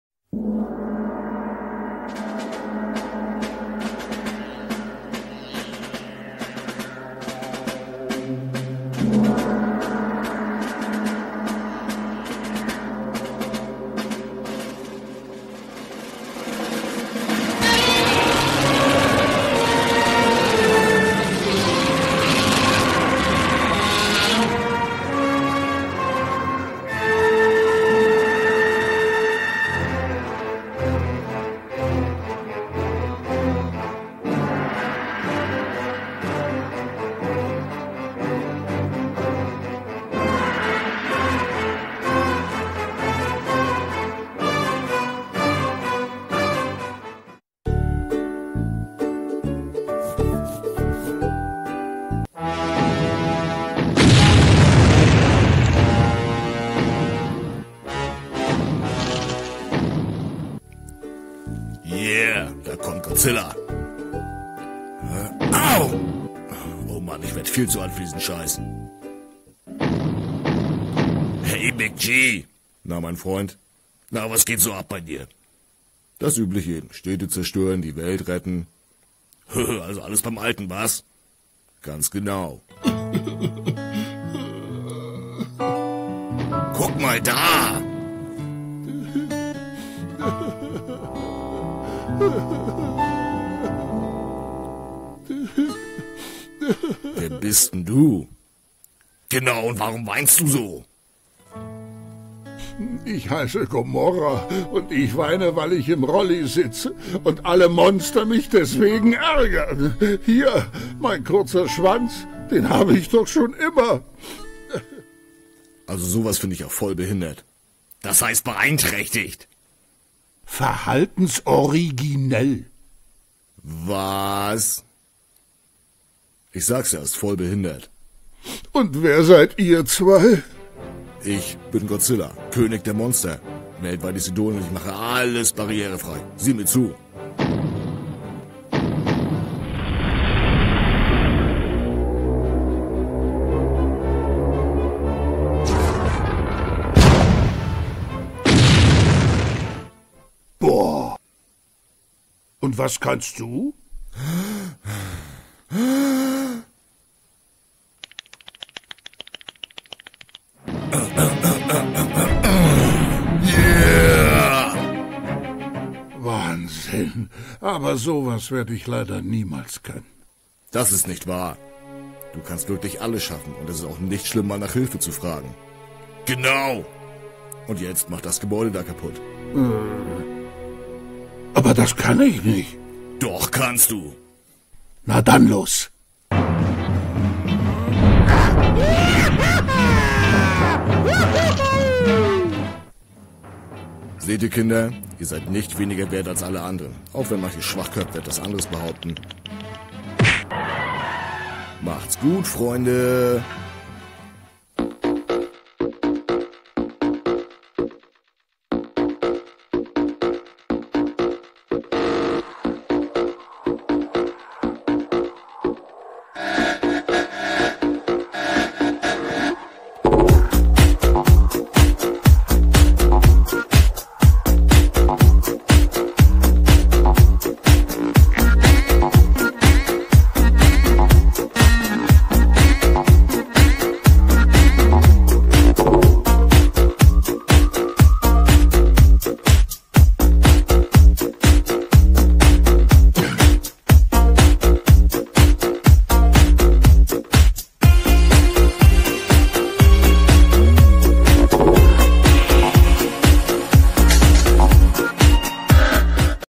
Stop Motion GODZILLA gegen Diskriminierung von Behinderten
Godzilla und sein bester Freund Gamera sind gegen die Diskriminierung von Behinderten. Sie treffen auf das Monster Gomora, welches ohne Schwanz geboren wurde und dadurch im Rollstuhl sitzt. Monster ohne Schwanz können nämlich die Balance nicht halten und deswegen auch nicht laufen.
Dieser stopmotion Clip war ein Schulprojekt mit der Klasse 7a vom Förderzentrum für Behinderte in Schwerin. Klassenlehrer Till Bamberg und seine Schüler haben das Set gebaut und zeichneten für die Effekte verantwortlich und animierten mit mir zusammen. Zudem spricht die dt. Synchronsprecherlegende Roland Hemmo (Benjamin Blümchen, Dragonball, Harry Potter) unser Monster Gomora. Vielen Dank Herr Hemmo 😊